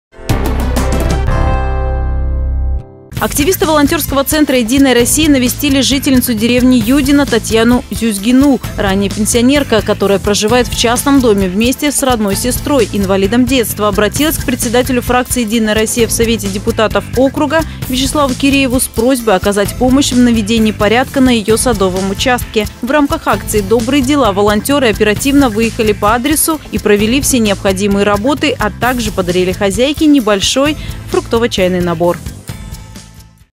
От всей души
Активисты Волонтерского центра «Единой России» навестили жительницу деревни Юдино Татьяну Зюзгину. Ранее пенсионерка, которая проживает в частном доме вместе с родной сестрой – инвалидом детства, обратилась к председателю фракции «Единая Россия» в Совете депутатов Одинцовского городского округа Вячеславу Кирееву с просьбой оказать помощь в наведении порядка на ее садовом участке.